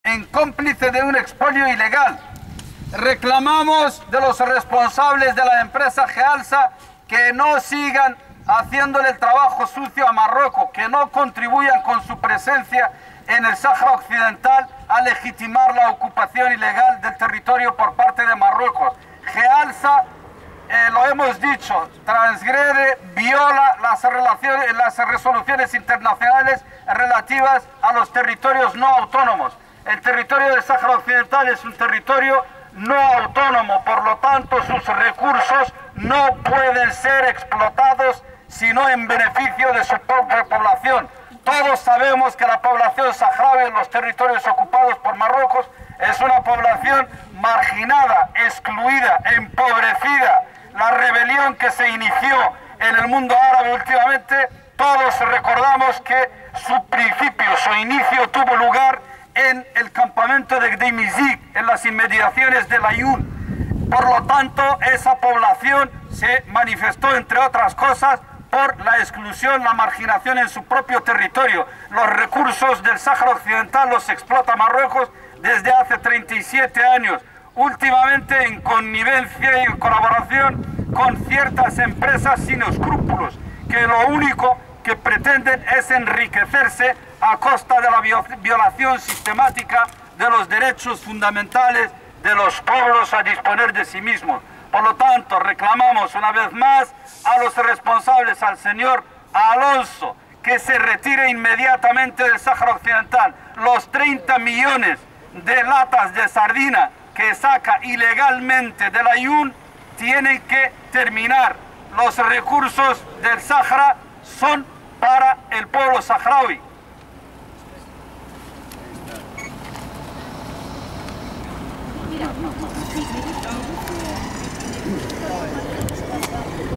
Protesting Jealsa: Spanish firm in occupied Western Sahara
Pro Saharawi demonstrators protest at the facilities of Spanish firm Jealsa, in the small town Boiro, Galicia, 27 July 2011. Jealsa has a factory in El Aaiún, capital of occupied Western Sahara. and makes bussines with Morocco's regime.
Most of the people employed by the firm are Moroccan settlers.
Morocco partially occupied Western Sahara in 1975. The indigenous Saharawi people are marginalised in their homeland, by the Moroccan state and the international firms.